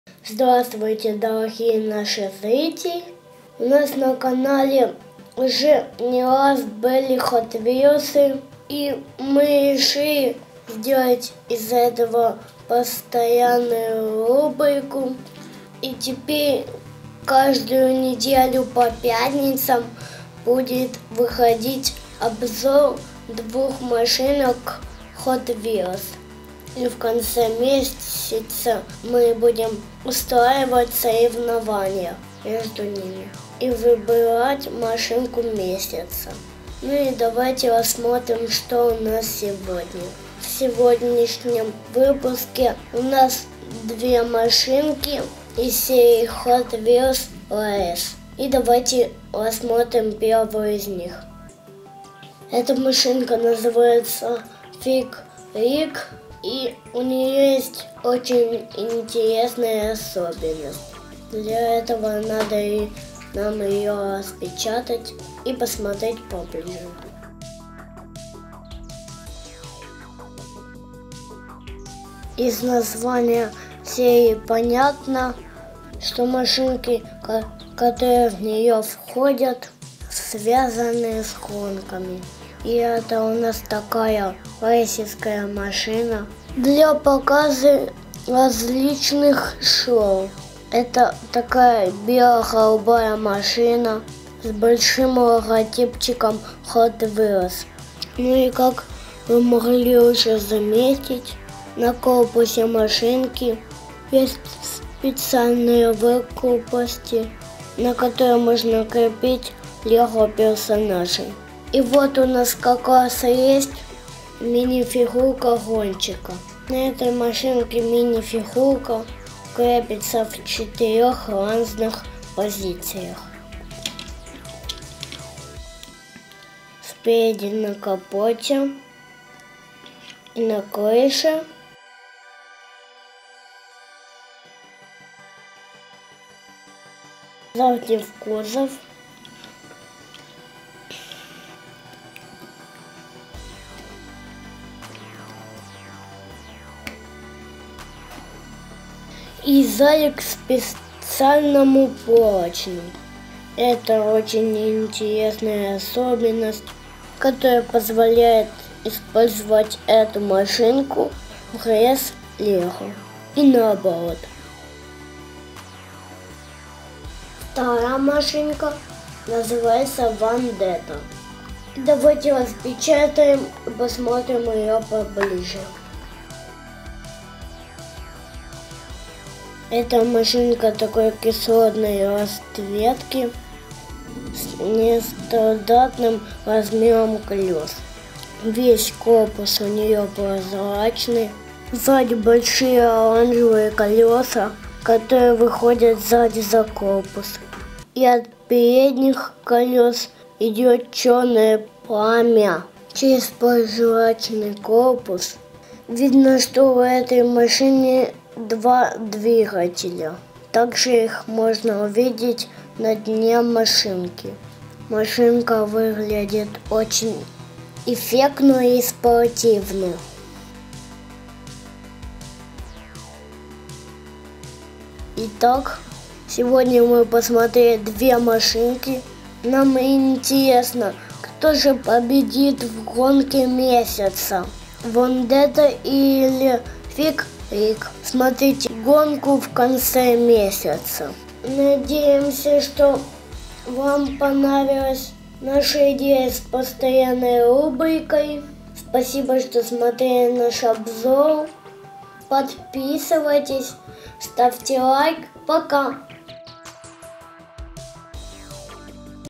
Соревнования Hot Wheels 1. HW Race (FigRig & Vandetta).
Новая постоянная рубрика с ежемесячным соревнованием в конце месяца.
Да победит сильнейший!